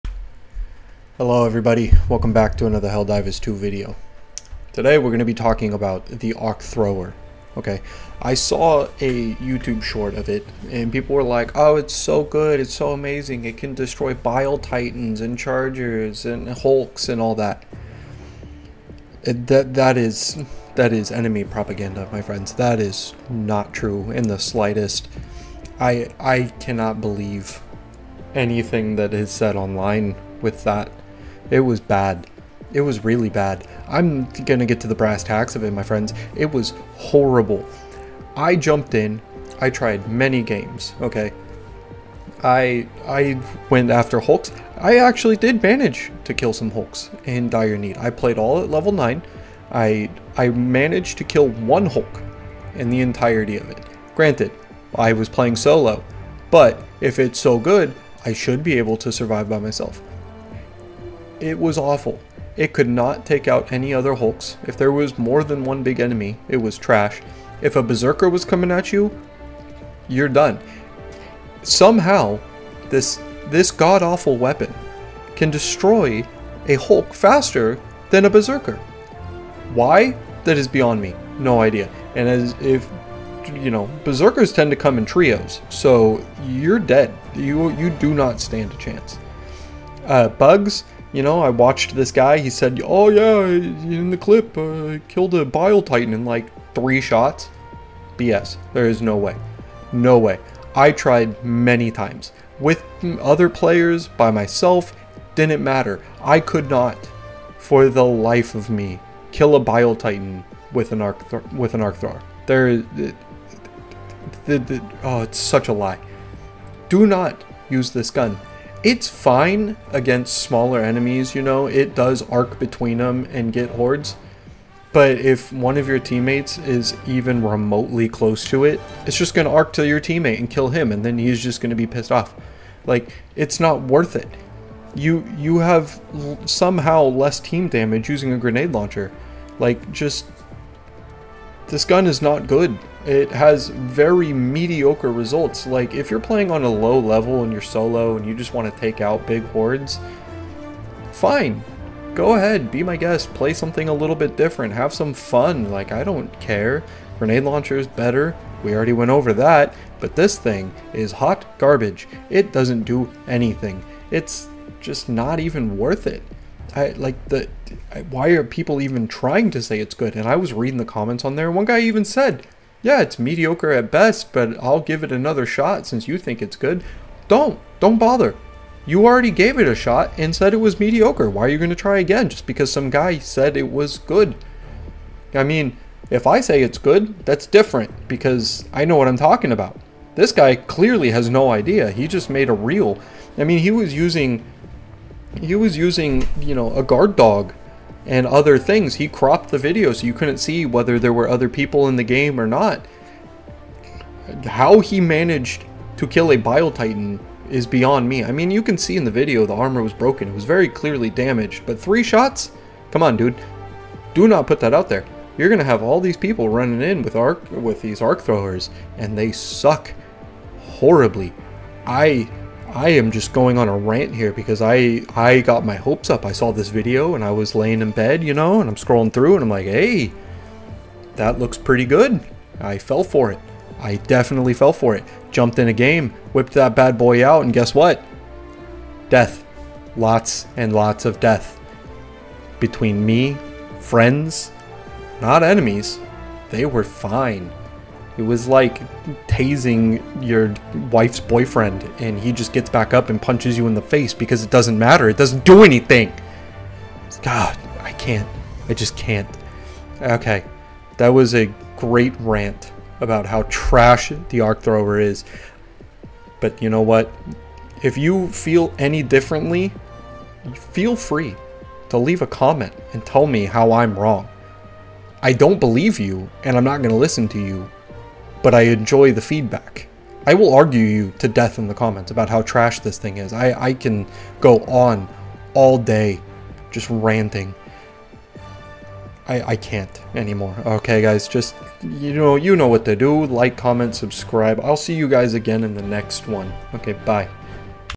Why Are People Saying This Is META! (Arc Thrower Rant!)
Hello everyone and welcome back to the WORST channel on YouTube, I'm your host DannyFTW7. Today I get heated over a video I saw here on YouTube, saying that the Arc Thrower is good. Guess what? It's not. I will argue everyone until I die about how useless this thing is. No one and I mean NO ONE can convince me otherwise!